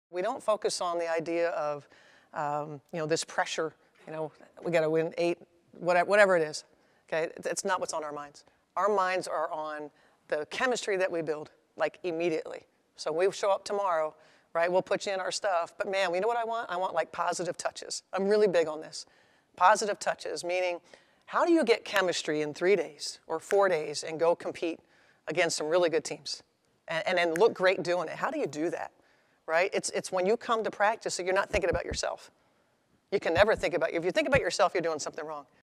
Head Coach Cheryl Reeve addresses Team USA on building chemistry
Head Coach Cheryl Reeve addresses Team USA on building chemistry during a preliminary camp ahead of the World Cup qualifying tournament. 2/05/2022